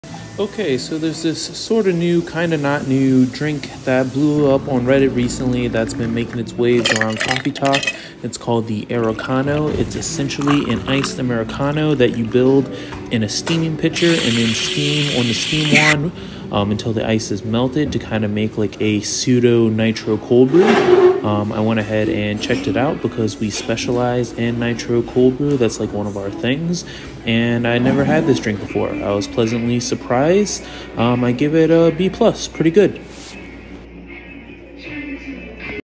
"Aerocano" in Under 40 Seconds
This "no really, but kind of new" simple drink has recently blown up on Reddit, and Tiktok. So we give it a try ourselves.